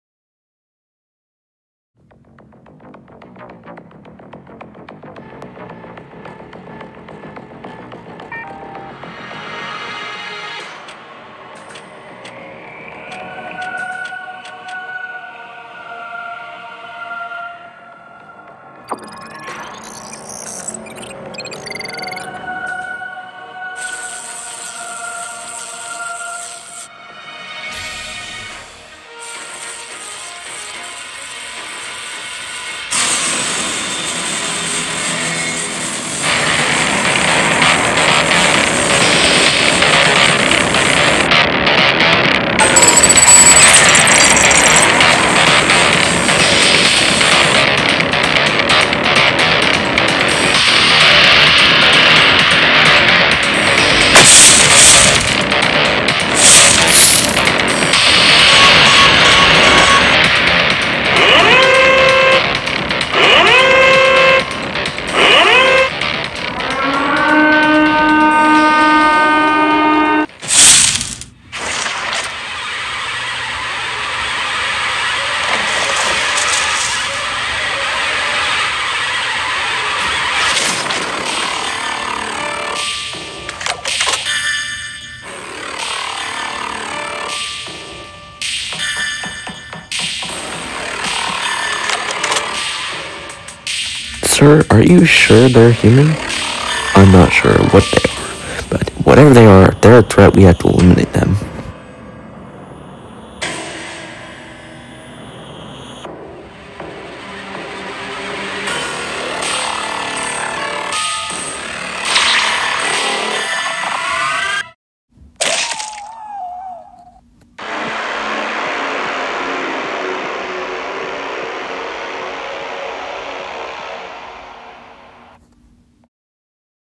IDS EXPERIMENT NO.122
The IDS (Inter. dimensional. Science-labs.) created a new type of Rifter portal called a tunnel. But when they use it something comes through.